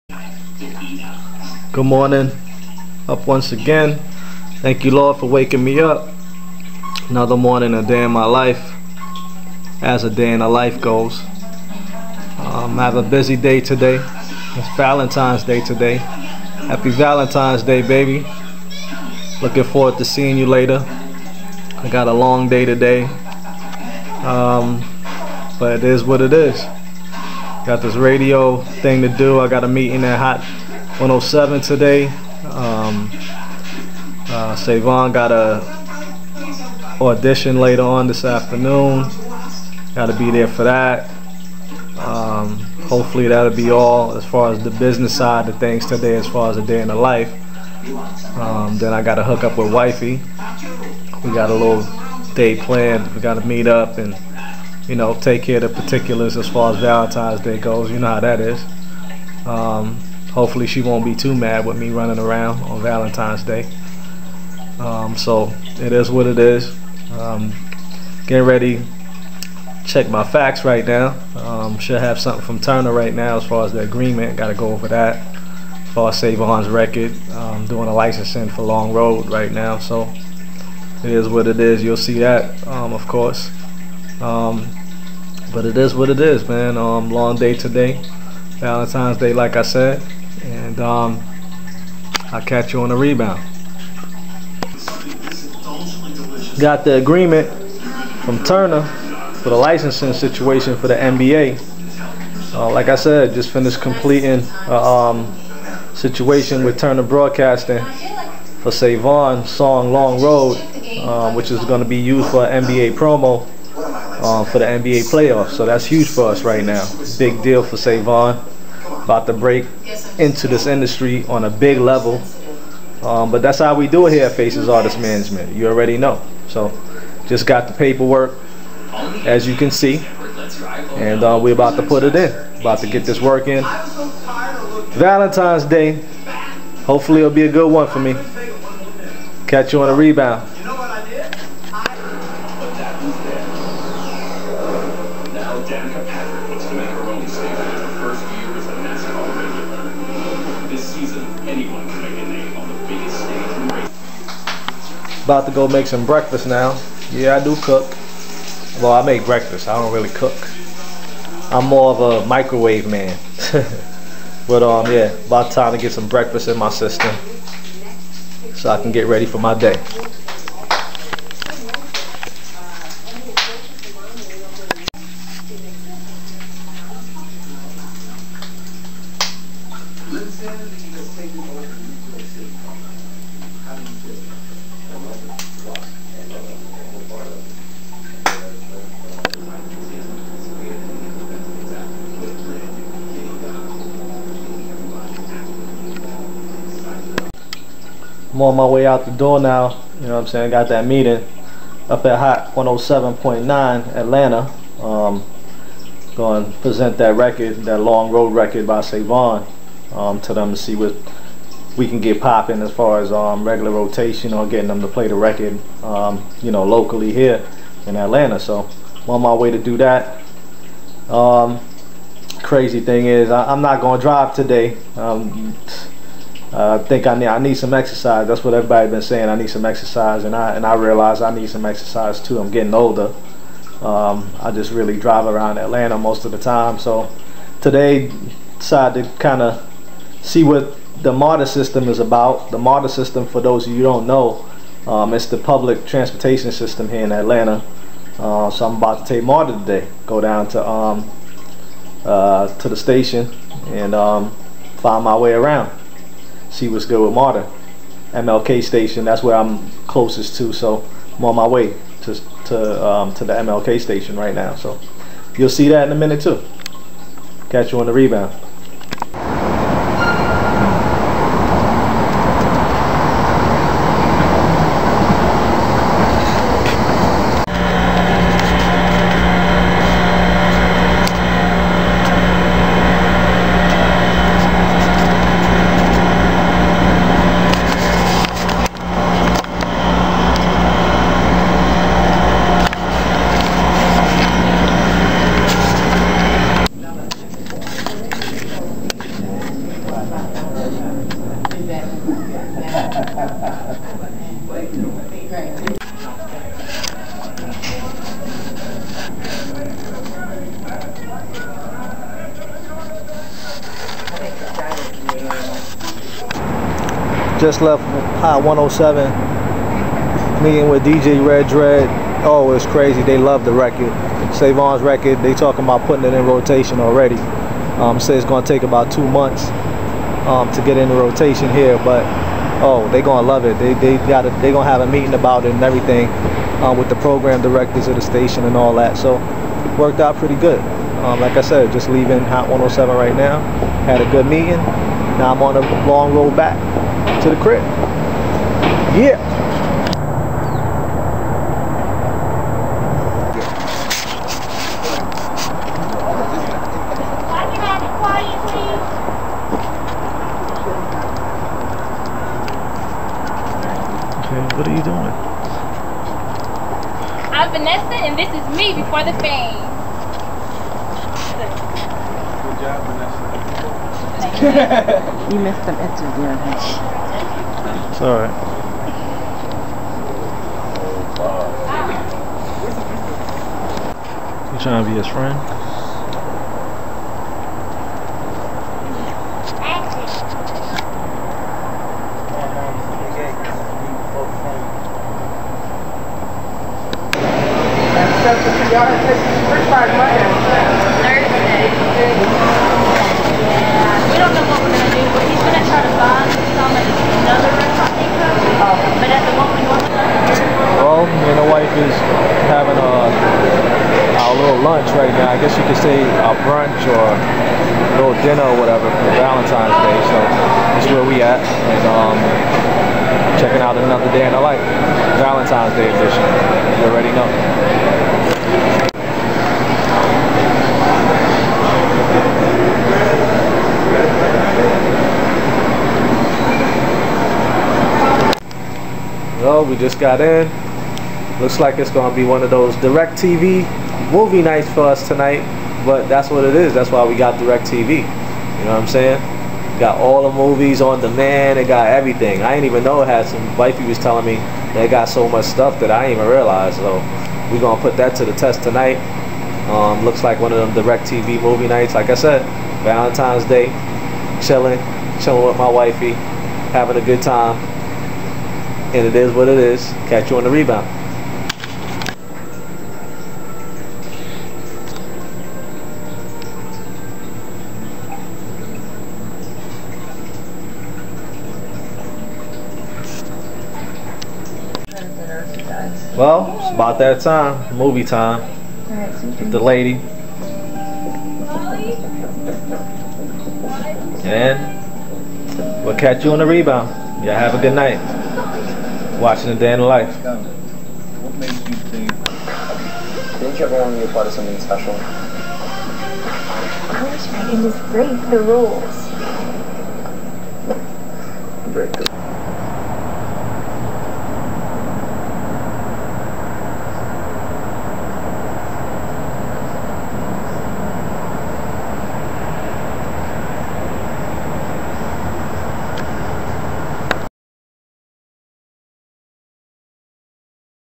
VALENTINE'S DAY EDITION - A DAY IN THE LIFE
A DAY IN THE LIFE OF CEO OF FACES INCORPORATED - STEVE RILEY AND A DAY IN HIS LIFE